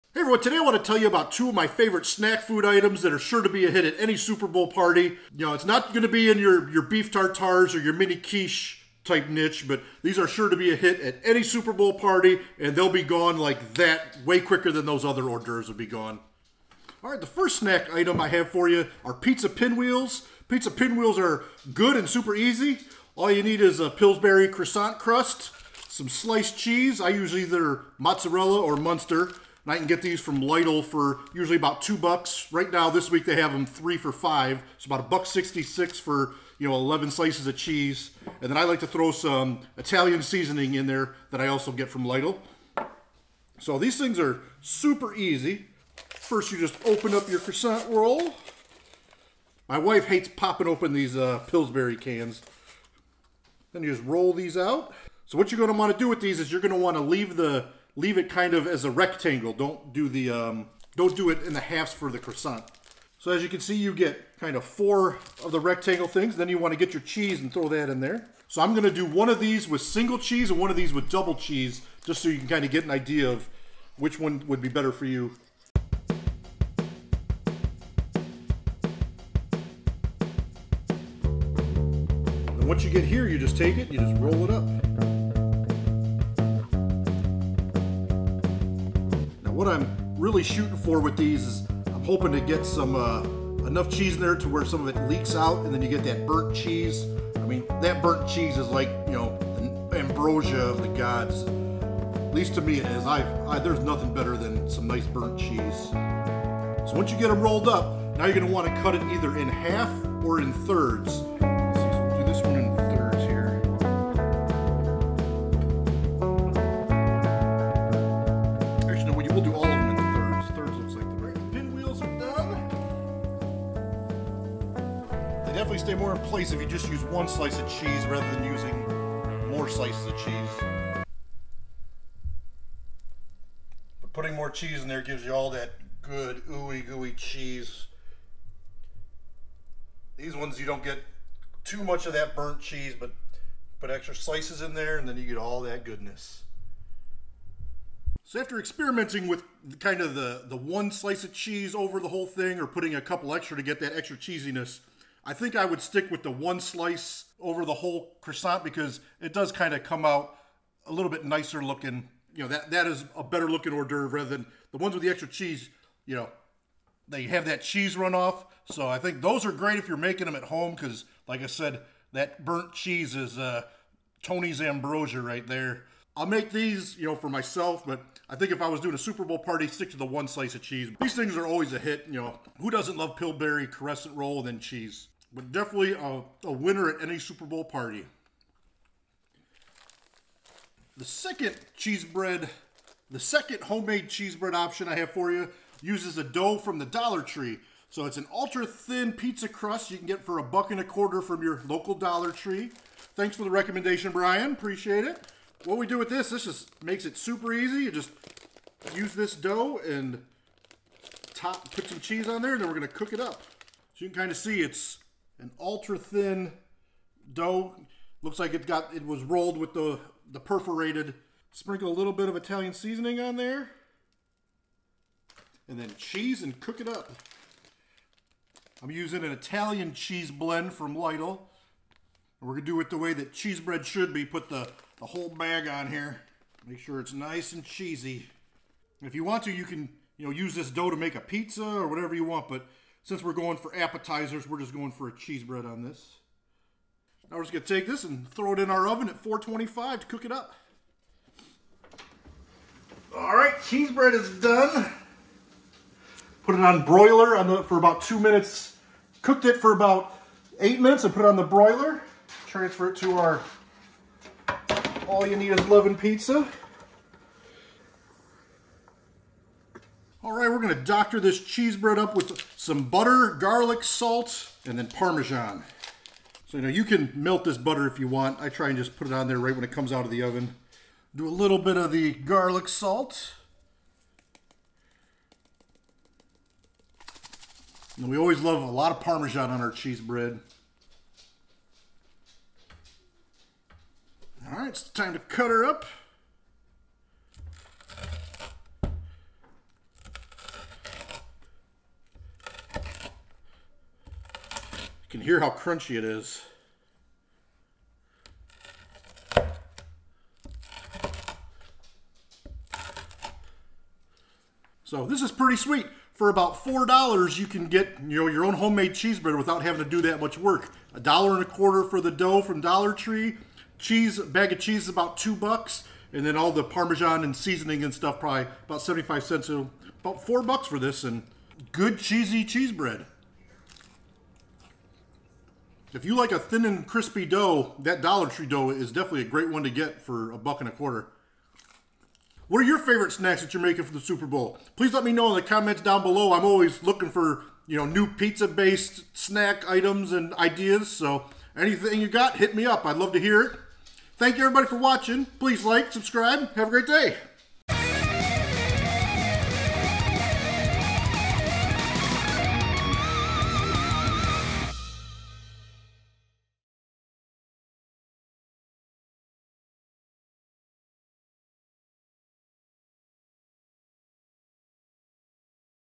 A Couple of Top Notch Economical Snacks for the Super Bowl
Here are a couple of easy snacks that do not require any mixing, and are great for your Super Bowl Party, or any other get together!